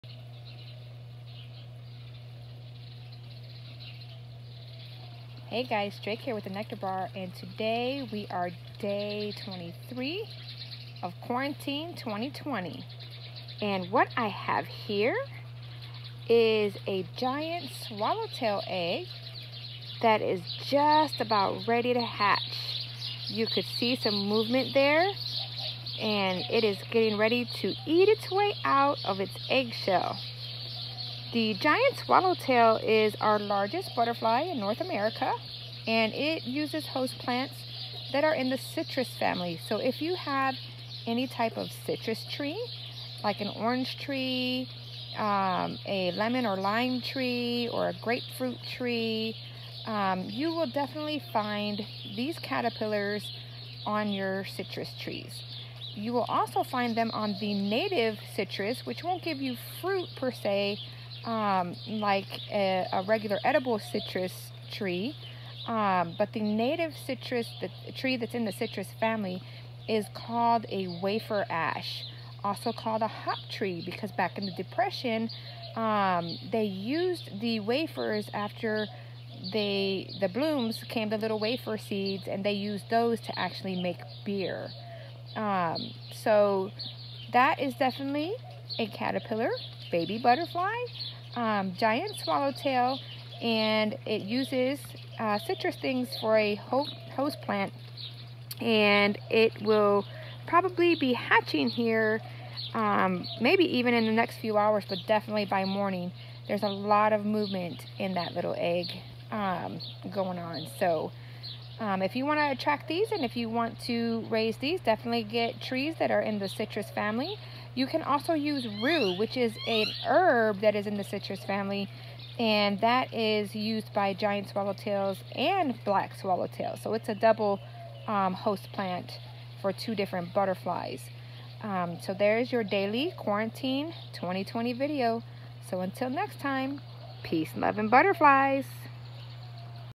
Giant Swallowtail about to hatch
Giant Swallowtails use plants in the citrus family. The Texas native tree in the citrus family is the Hop tree (Ptelea trifoliata). Other edible citrus include Lemon, Lime, Oranges, and grapefruit. Though the caterpillars are large (resembling bird poop), they won’t defoliate your tree. Unlike their other Swallowtail cousins. ✌🏼💝🦋🦋